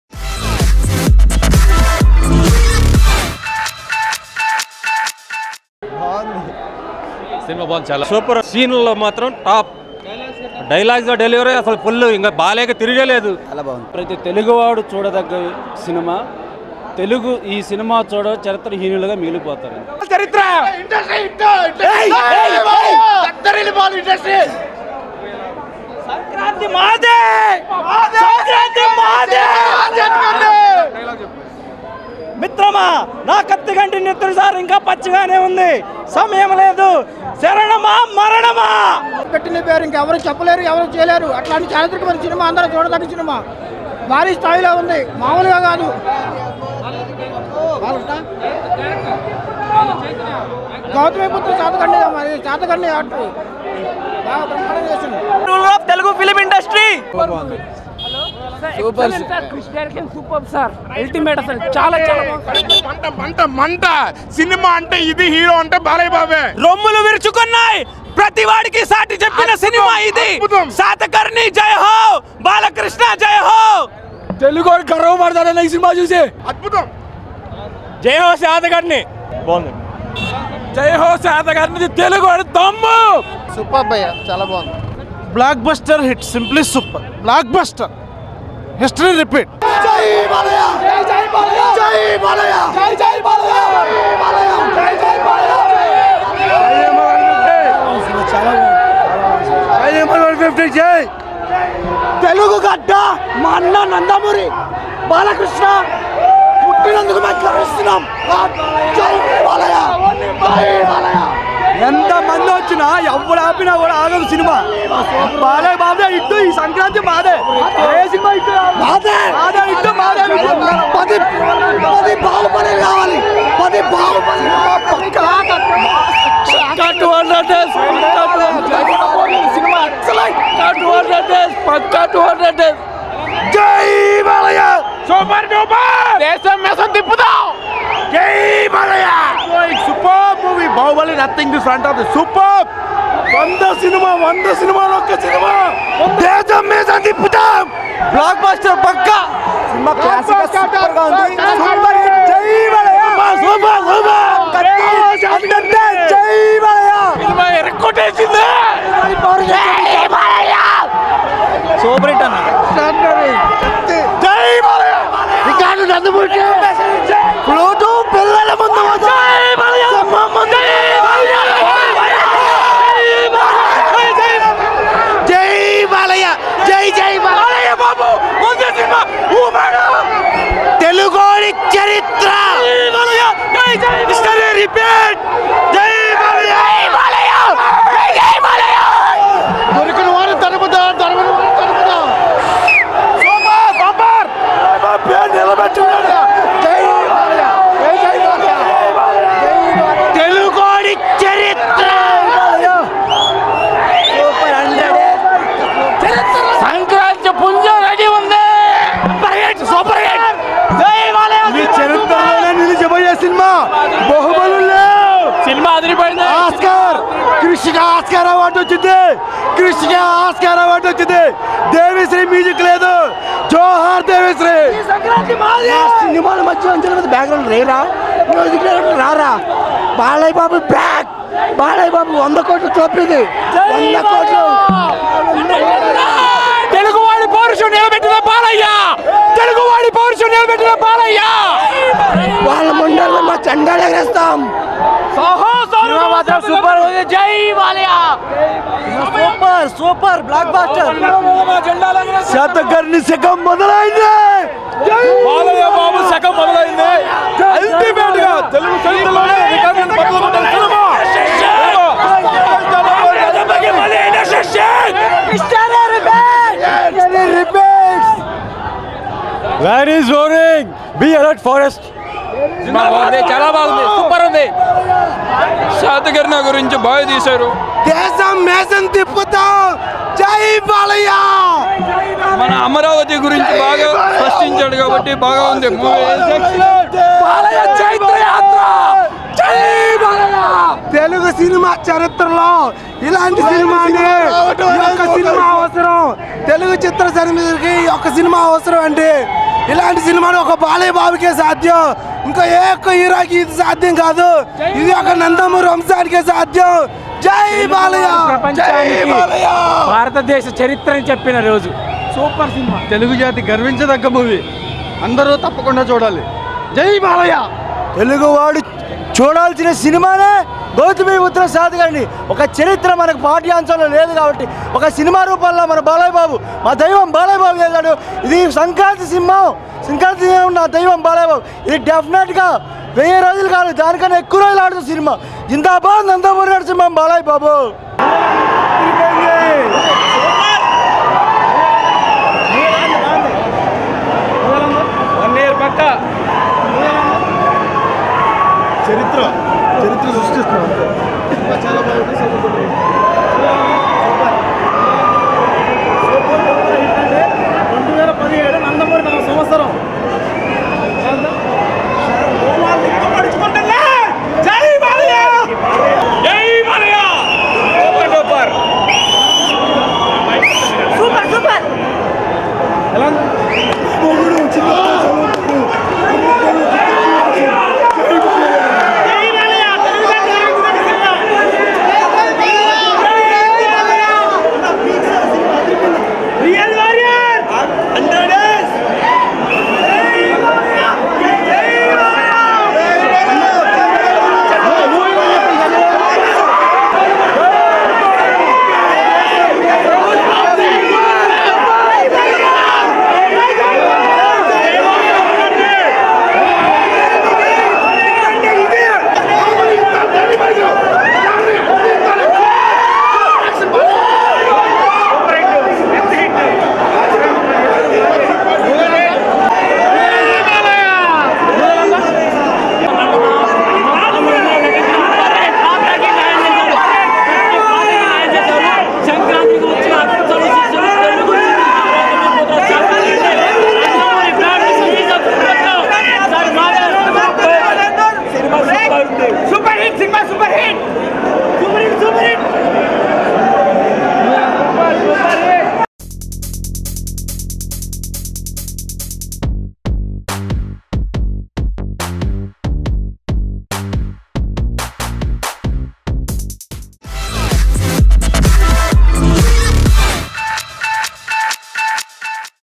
Gautamiputra Satakarni Public Talk-Response-Review ||Nandamuri Fans Hungama At Theatres -Public Talk
Watch Gautamiputra Satakarni Public Talk-Response-Review ||Nandamuri Fans Hungama At Theatres -Public Talk
GautamiputraSatakarni Movie Ft.Nandamuri Balakrishna, Shriya Saran, Hema Malini
Music composed by Chirantan Bhatt
The film is based on life history of 2nd century CE, Satavahana ruler Gautamiputra Satakarni